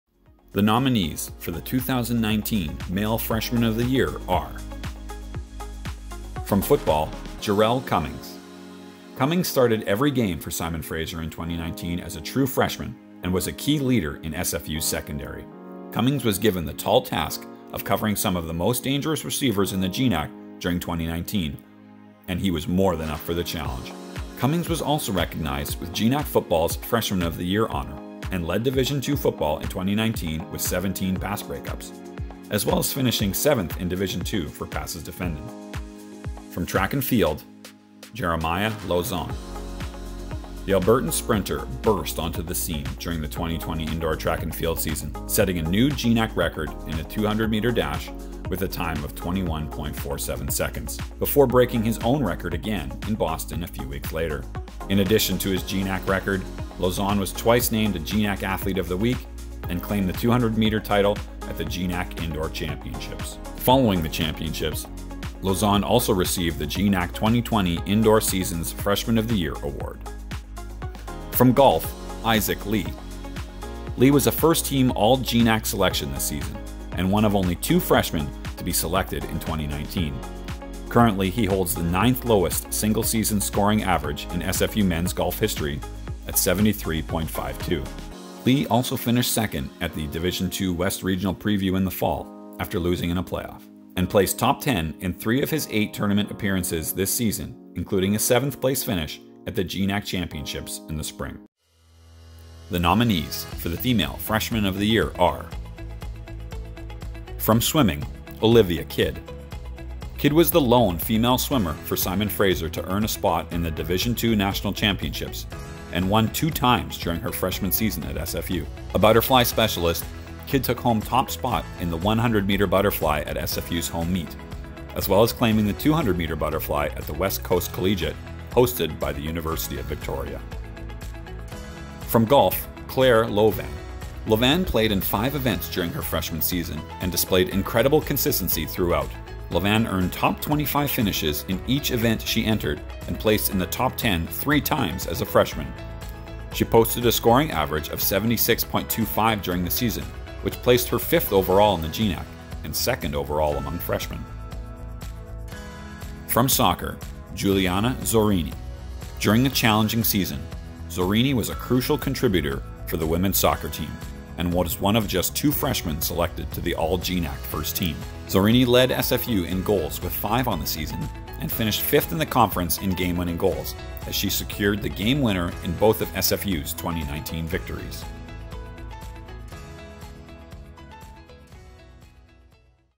SFU Athletics: Male and Female Nominees for Freshman Athlete of the Year
Here are your finalists for Freshmen of the Year! RepTheLeaf 🍁